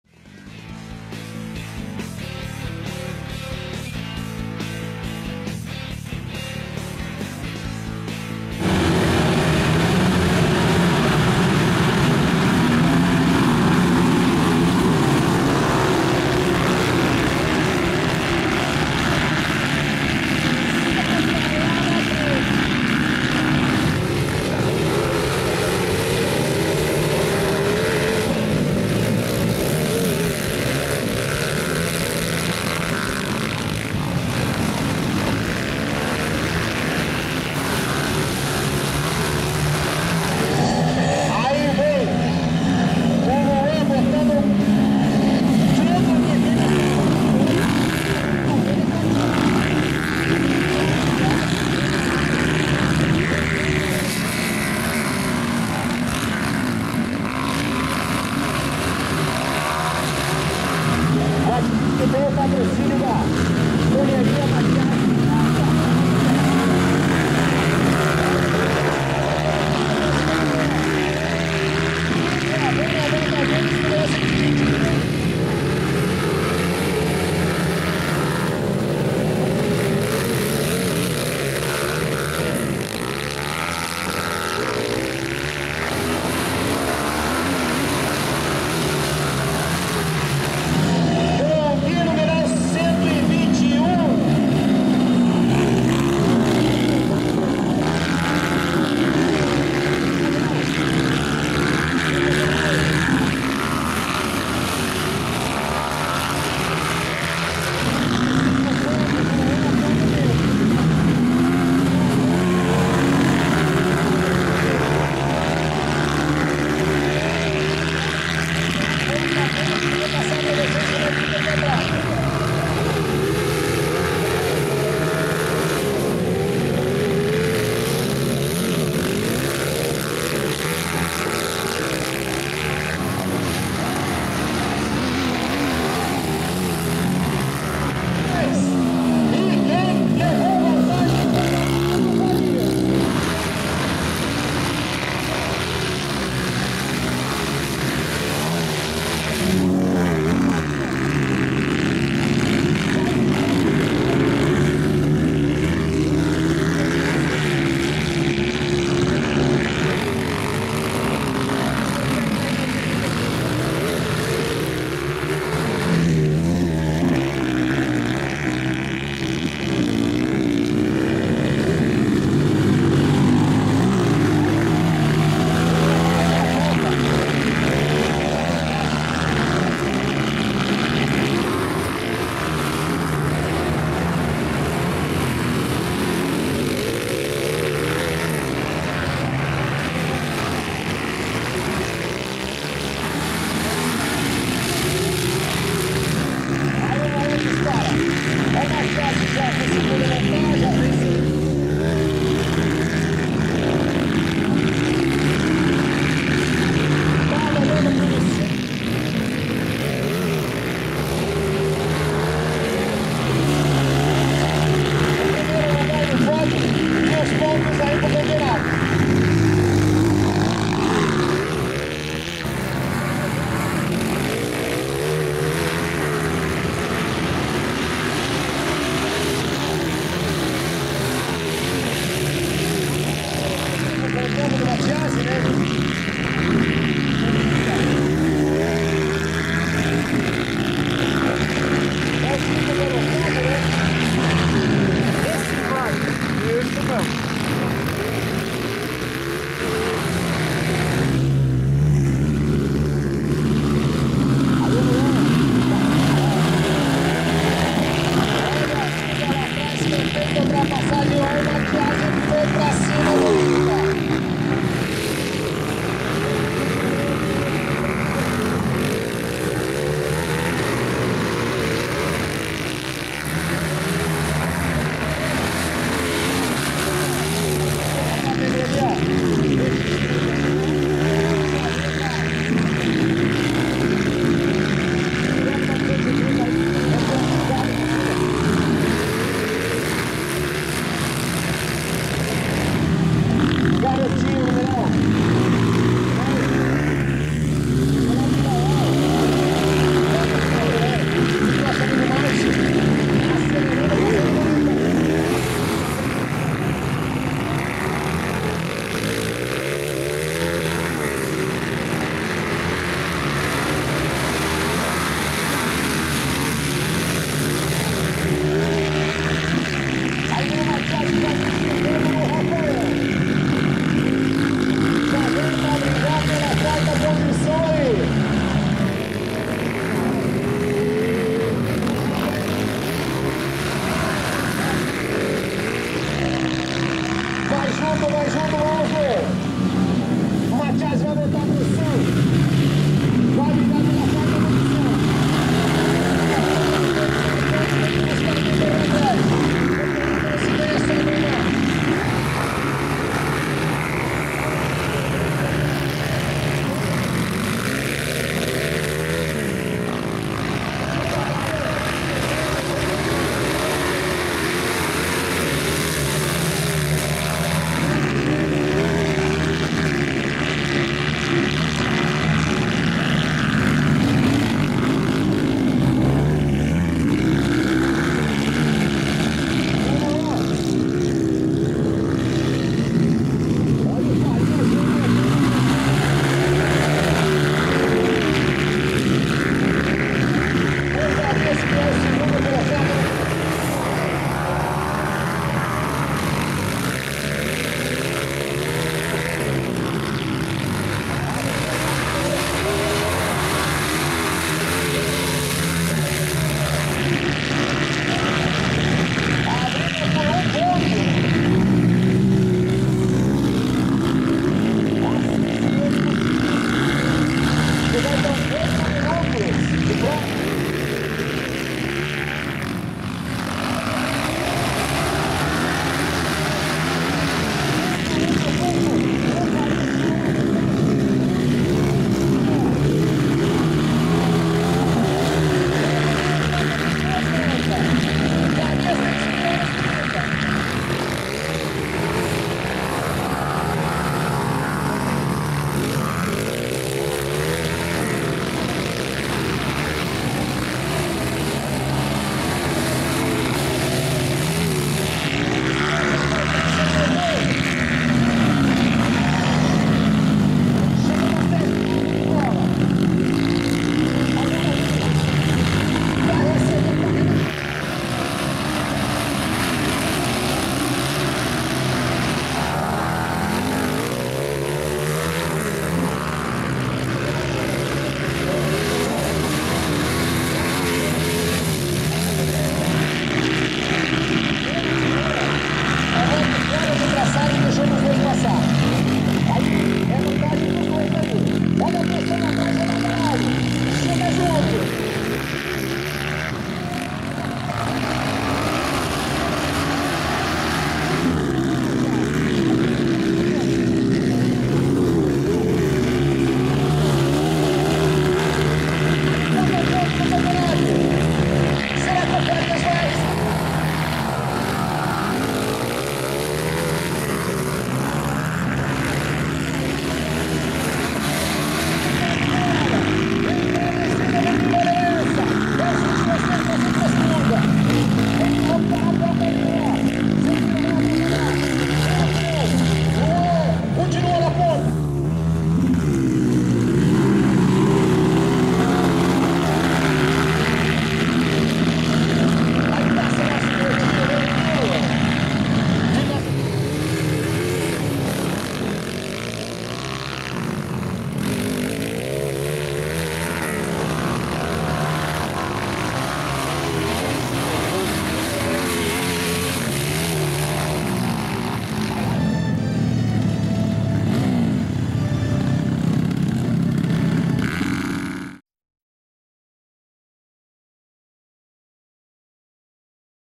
Velocross na Lama - Tombos, Ultrapassagens e boas disputas em Massaranduba SC
Velocross na Lama - Tombos, Ultrapassagens e boas disputas na 7a etapa da Copa Brasil de Velocross em Massaranduba SC

17 e 18/11

Categoria Nacional 230cc